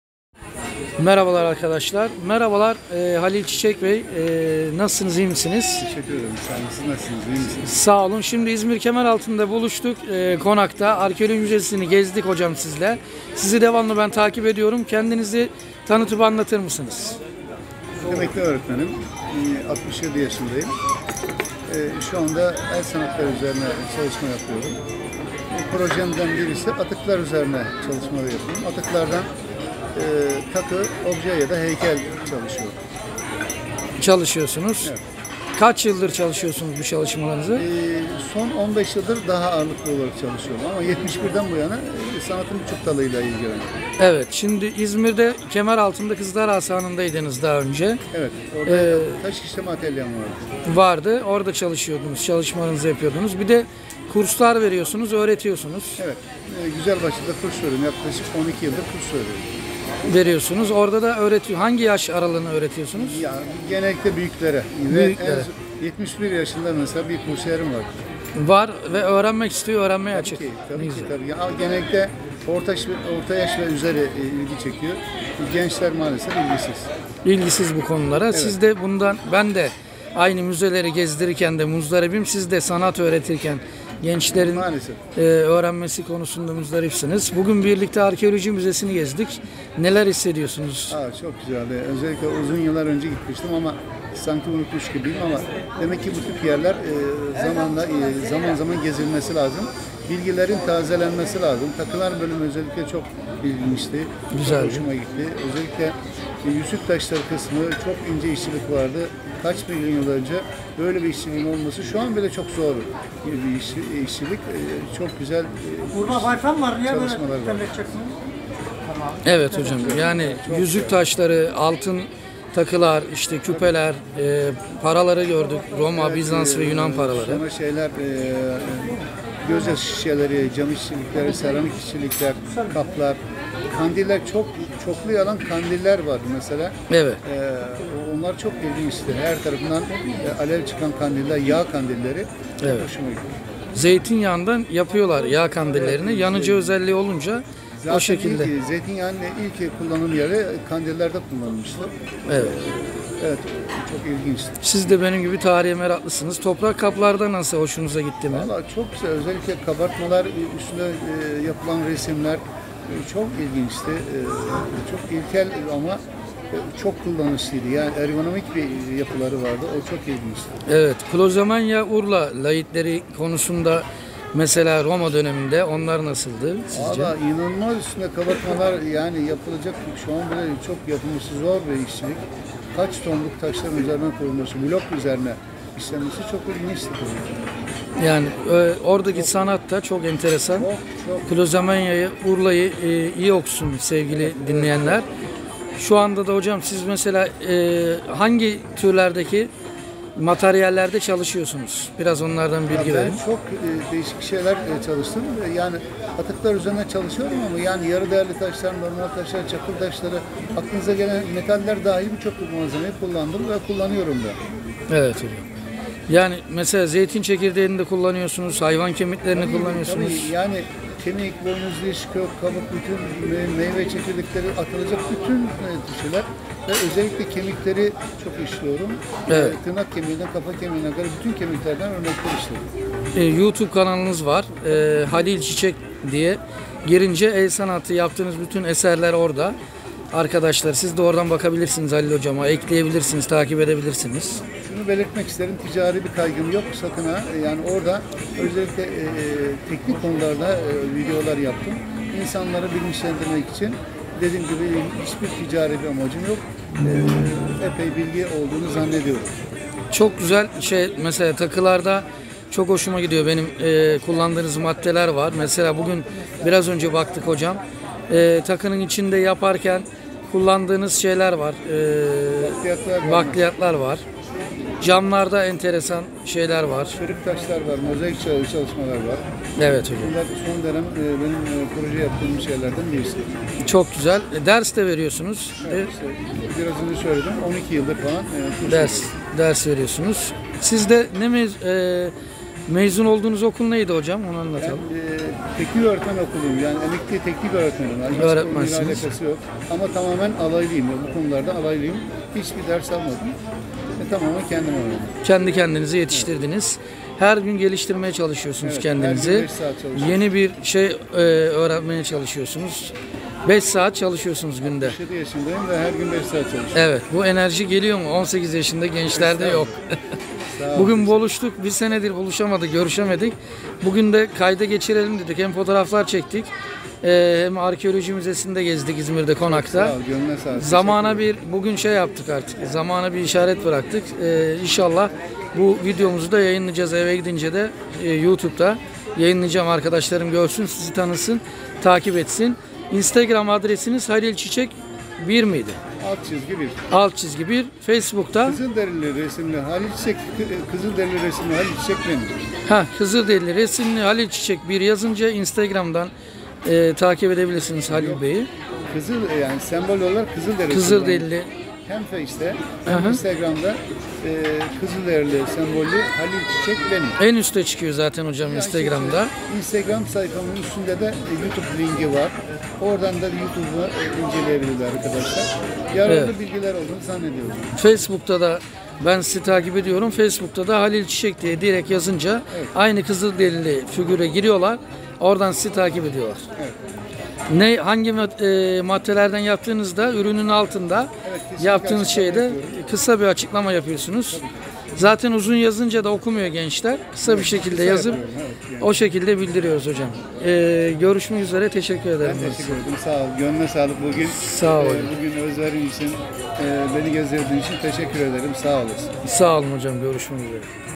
Atıklarla Takı obje ve heykel yapım ustası Emekli Öğretmen Halil Çiçek ile Röportaj
Atıklarla Takı obje ve heykel yapım ustası